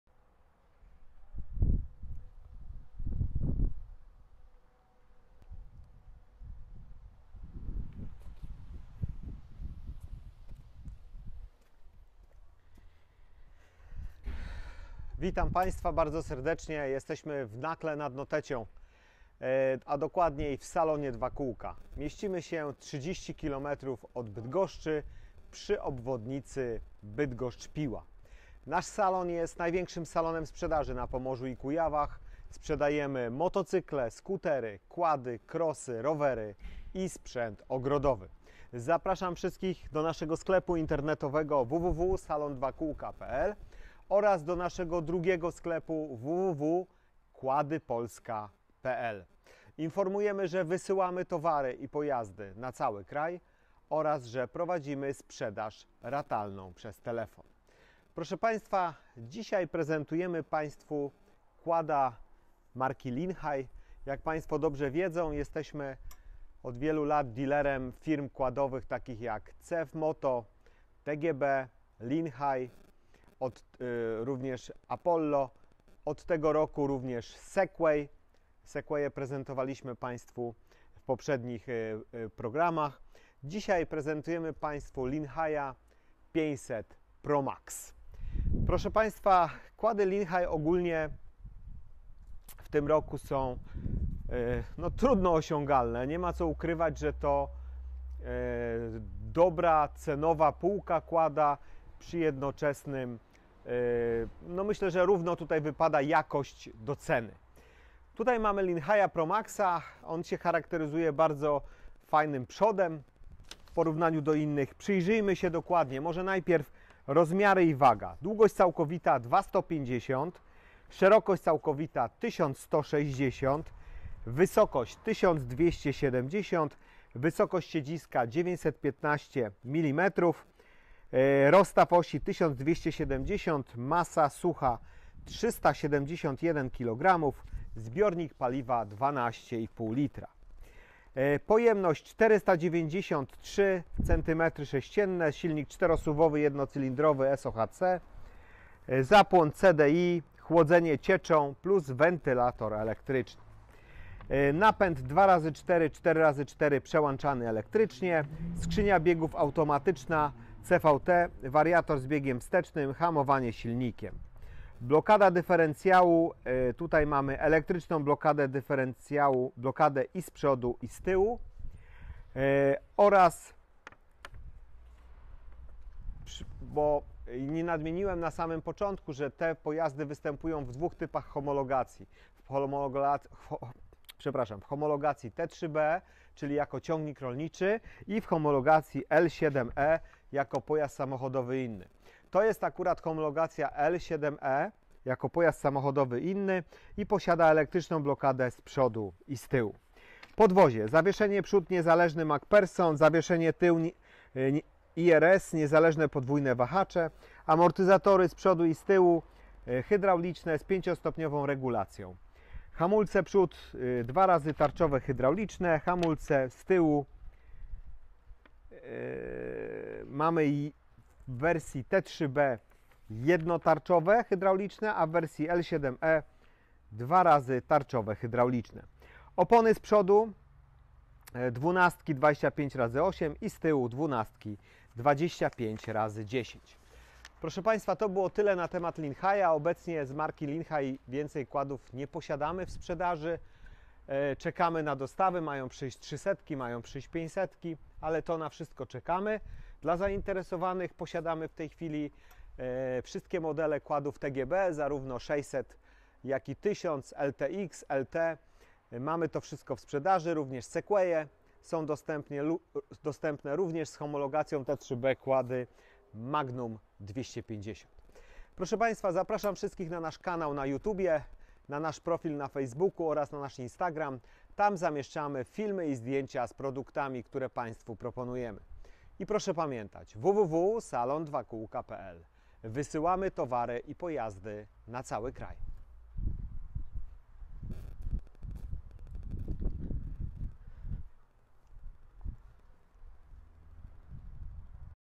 Quad Linhai 500CC ATV500 Pro Max E7L 4X4 2020 - prezentacja quada
KUP TERAZ!

Nowoczesny, przyciągający uwagę quad wyposażony w oświetlenie full-LED. Linhai 500 ProMax jest doskonale przygotowany do wykonywania wszelkich prac w gospodarstwie, lesie, czy ogrodzie. Podczas podróży, w trudnym terenie i w codziennym użytkowaniu docenisz sprawne i komfortowe zawieszenie z niezależnym zawieszeniem kół, niezawodny silnik, elektrycznie przełączany napęd 2×4 / 4×4 z blokowanymi mechanizmami różnicowymi oraz precyzyjne sterowanie.